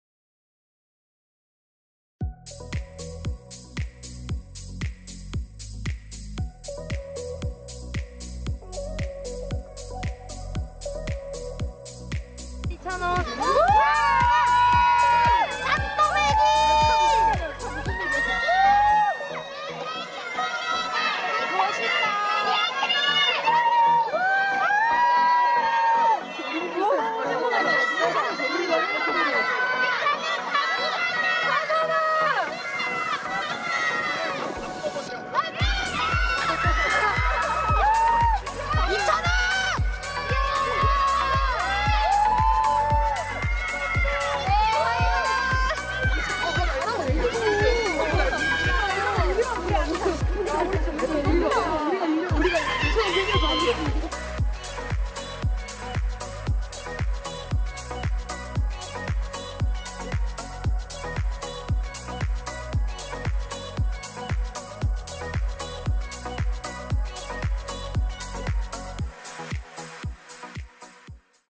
찬또배기 이찬원, 찬원마을 이장님 저녁 식사하셨어요? (TV조선 '사랑의 콜센타' 퇴근길)
5월 4일 저녁 인천광역시 중구 한 스튜디오에서 TV CHOSUN ‘신청곡을 불러드립니다 - 사랑의 콜센타’ 녹화를 마치고 미스터트롯 미 이찬원이 퇴근 하고 있다

미스터트롯, 사랑의 콜센타, 임영웅, 영탁, 이찬원, 김호중, 정동원, 장민호, 김희재, 사랑의 콜센터,찬또배기,찬원마을